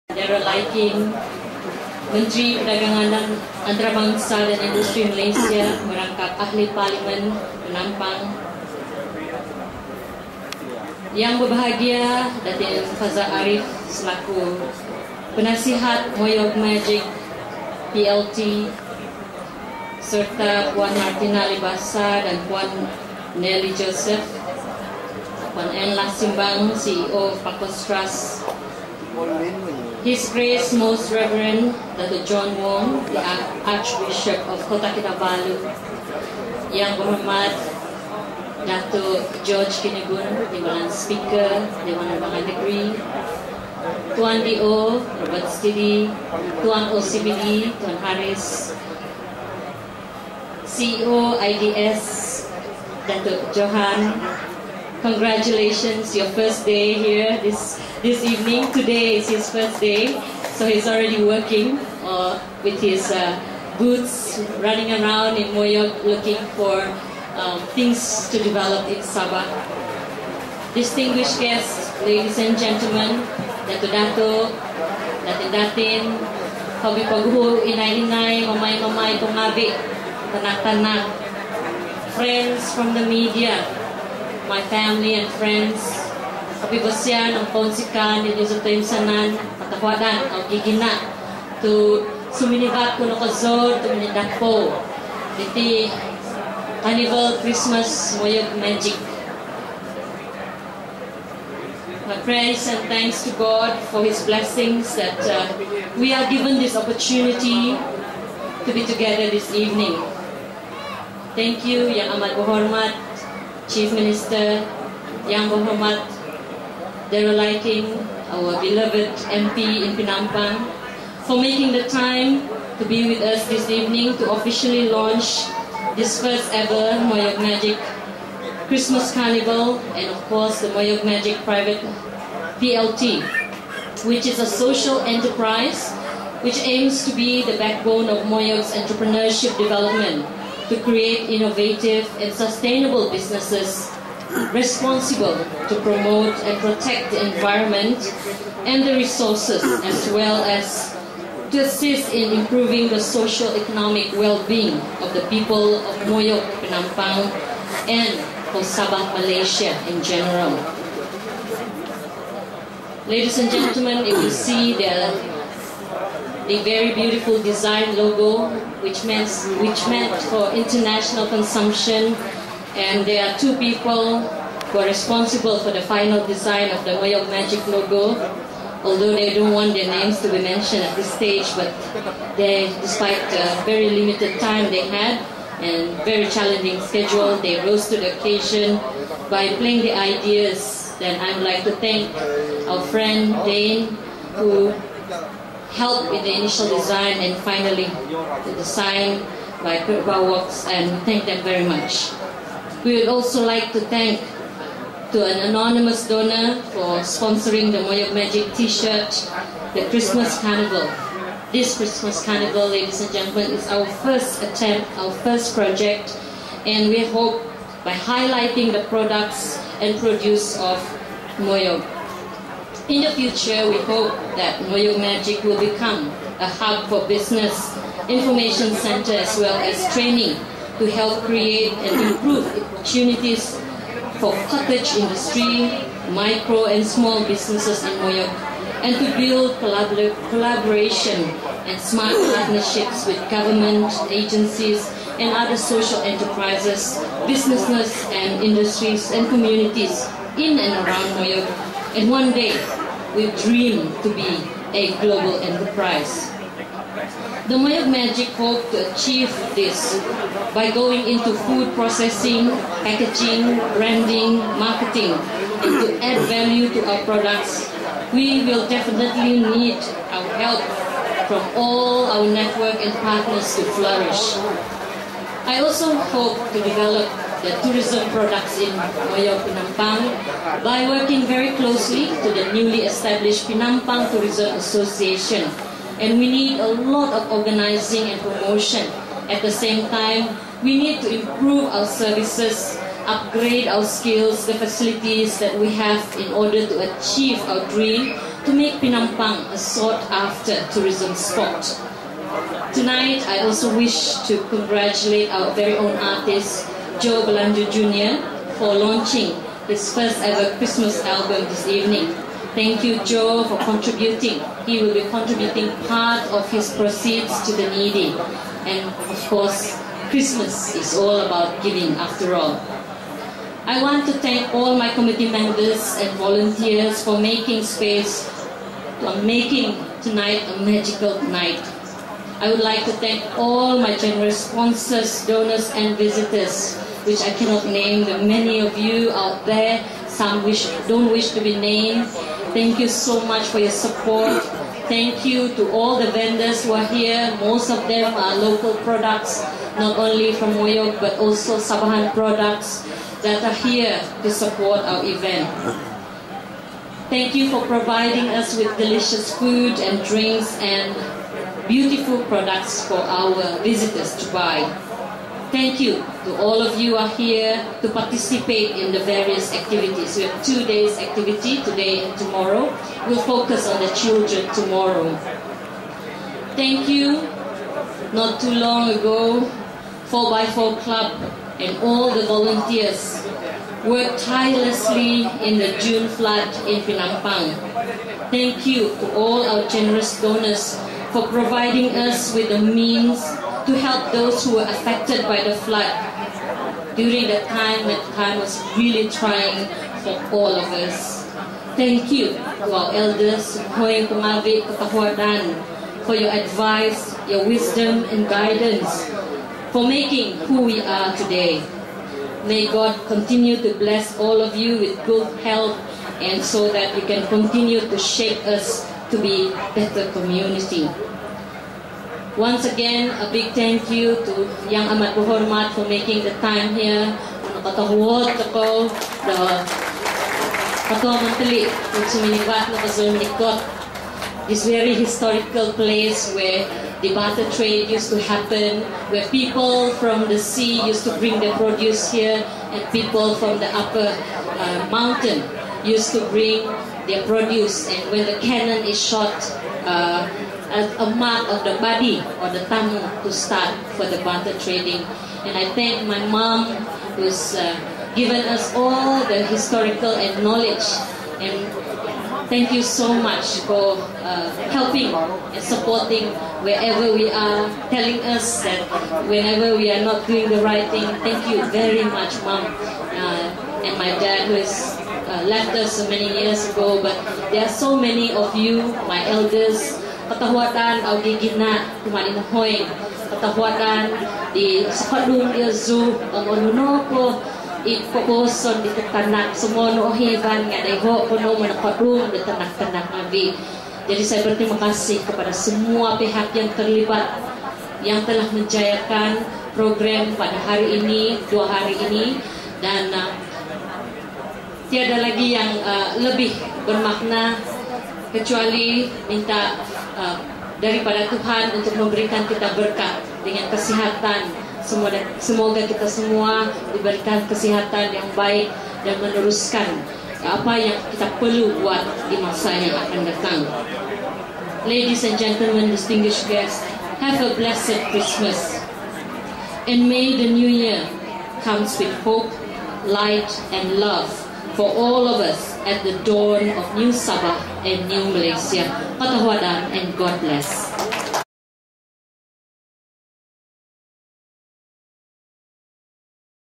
YB JENIFER LASIMBANG MSG INOBONG TAMU CHRISTMAS CARNIVAL 1 DEC 2018
Video taken with my own small sony handicam

AS per heading